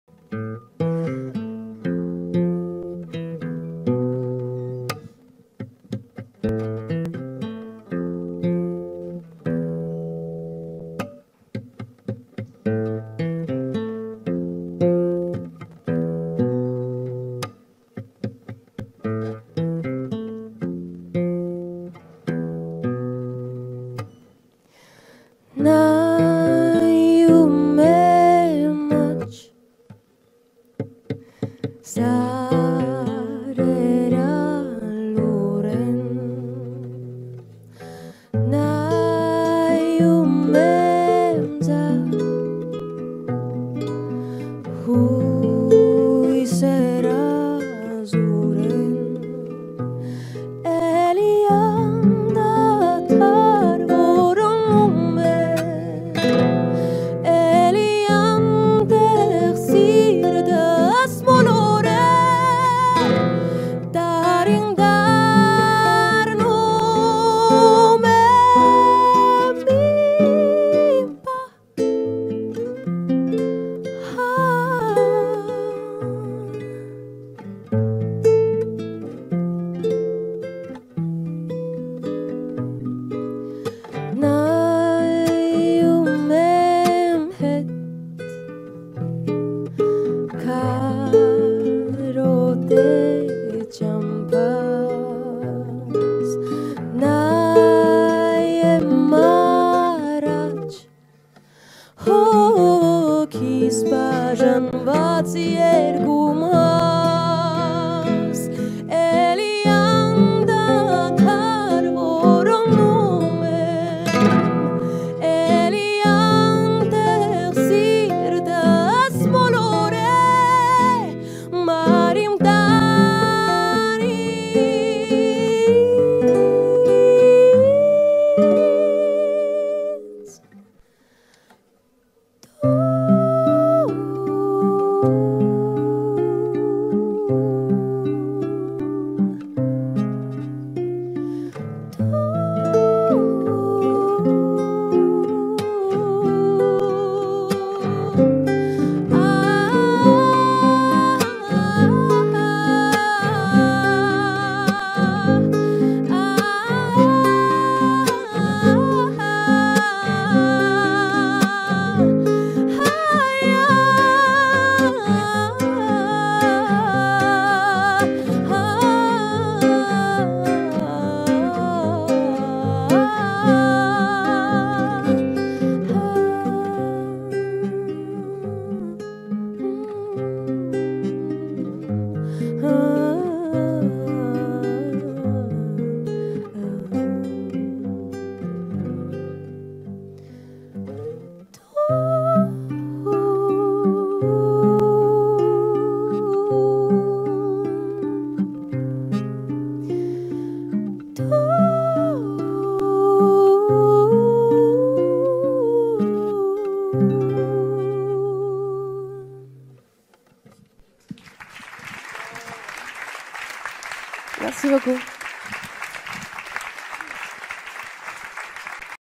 Ladaniva aux Suds, à Arles - apéro découverte - Espace Van Gogh 13/07/2021
Prise de son: La Radio des Suds